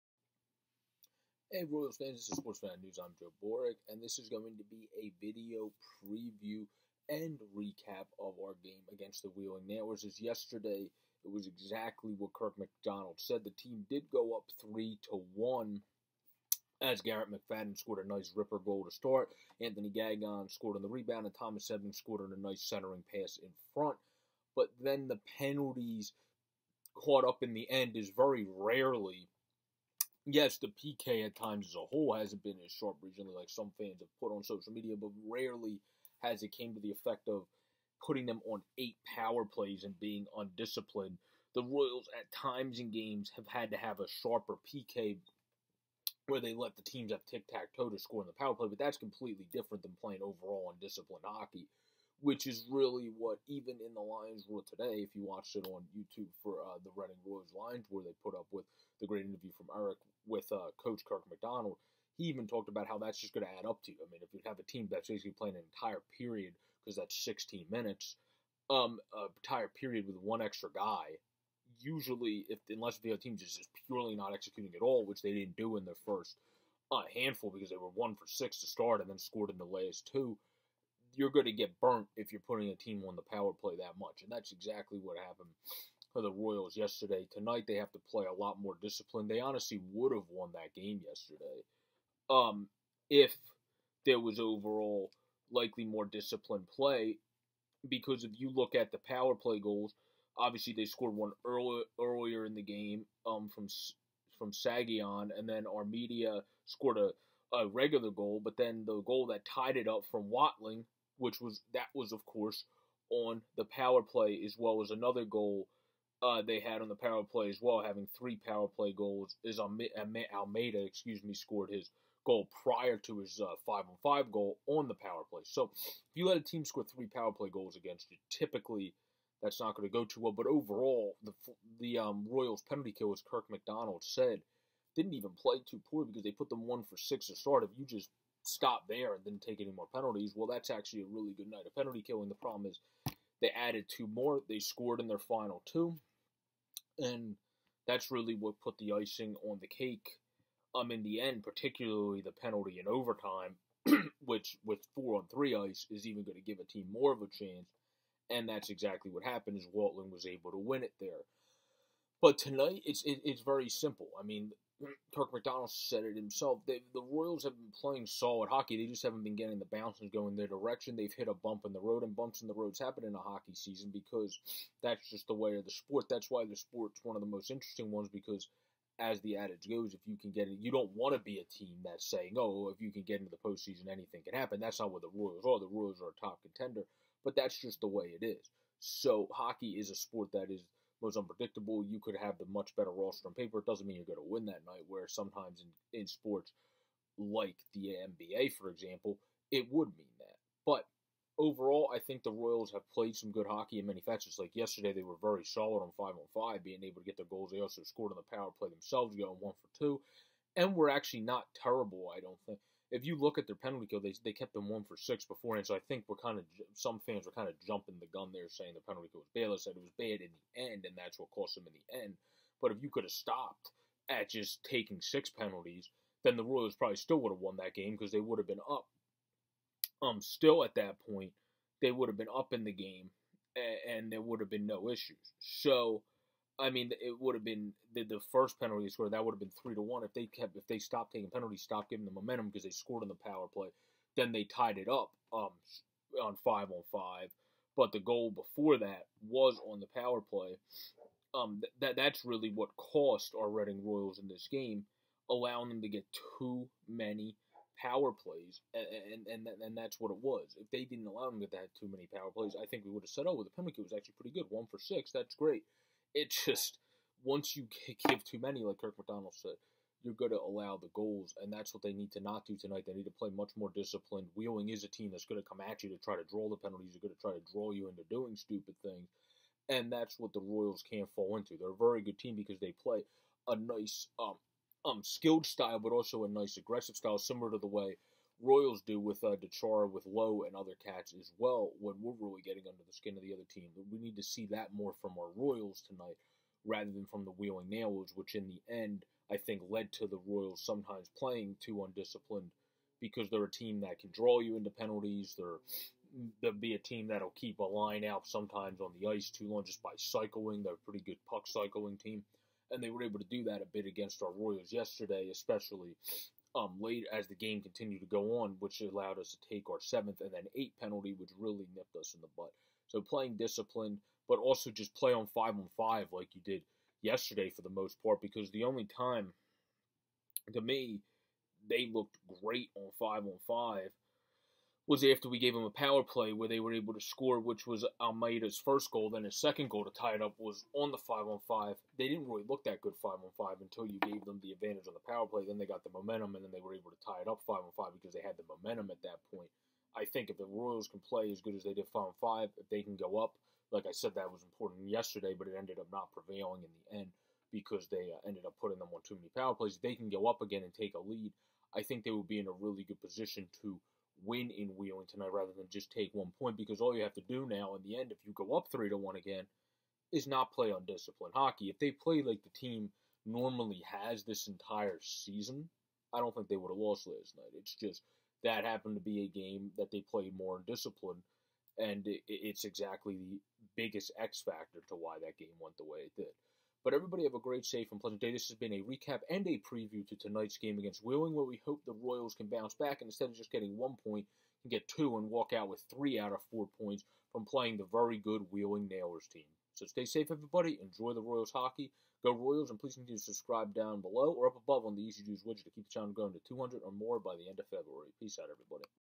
Reading Royals Look to Get Revenge After too Many Penalties Cost Them Last Night
In This one Pro Joe breaks down the Recent play of our Royals quickly and then goes over what the Royals need to do in his eyes to get revenge tonight in this Reading Royals Look to Get Revenge After too Many Penalties Cost Them Last Night preview and recap video. Thanks all and have a great day and enjoy the game.